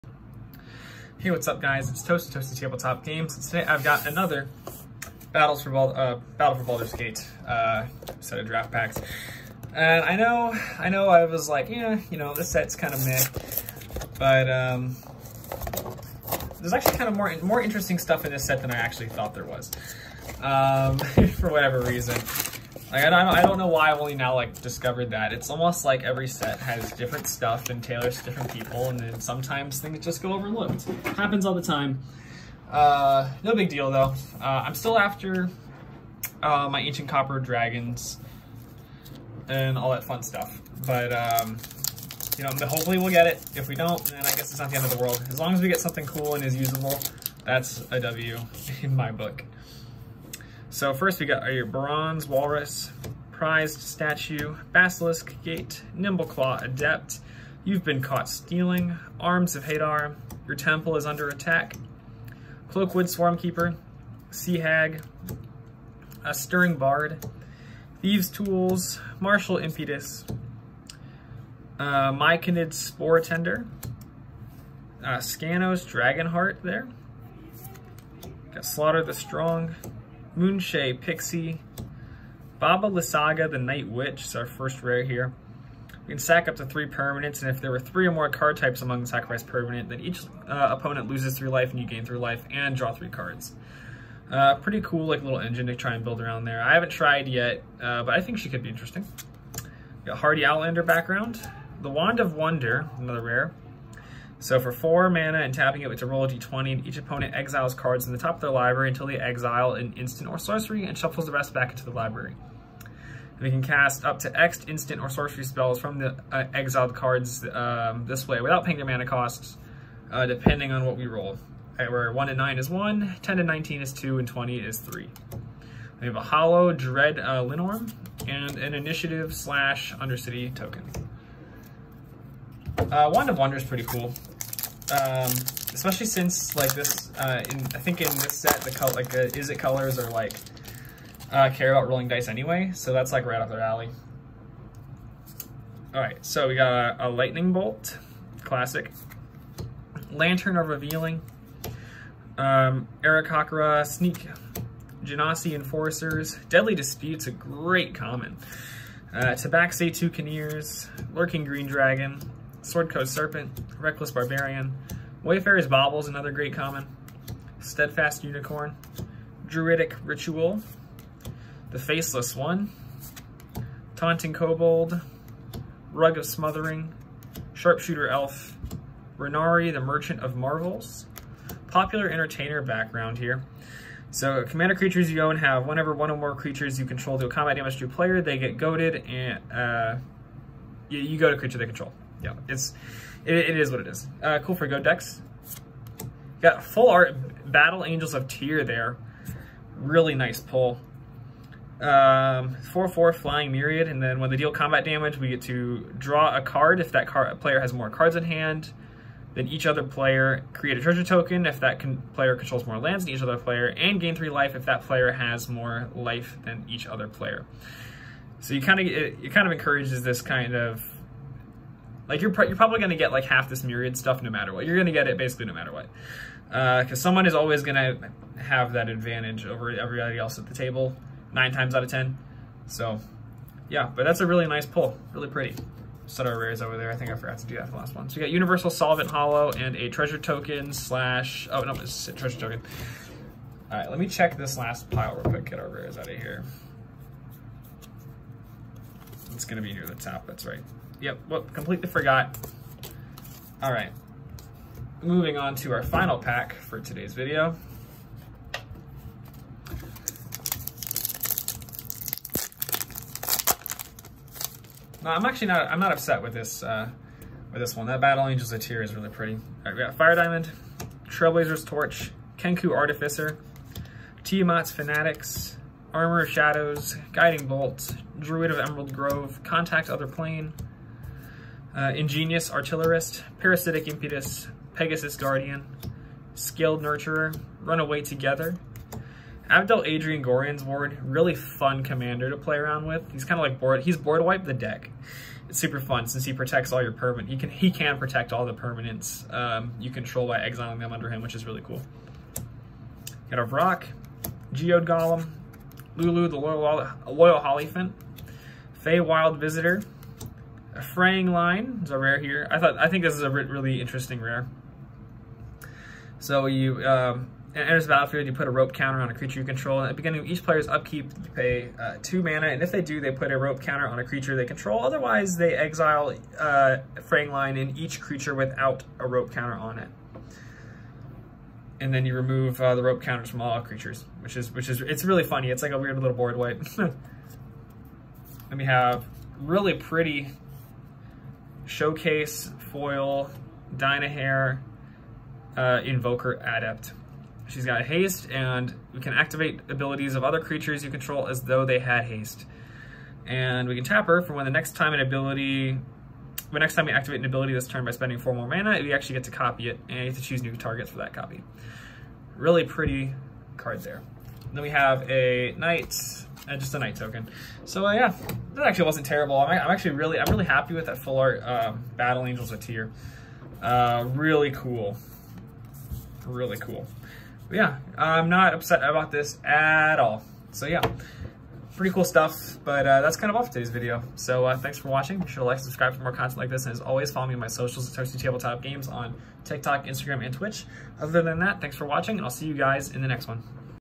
MTG Commander Legends Battle For Baldur’s Gate Draft Pack Opening
Pretty solid rares and mythics for todays video! I seriously doubt we made money back on it, but when is that ever the case with this set? Nonetheless, solid pulls, and I really hope you guys enjoy the video!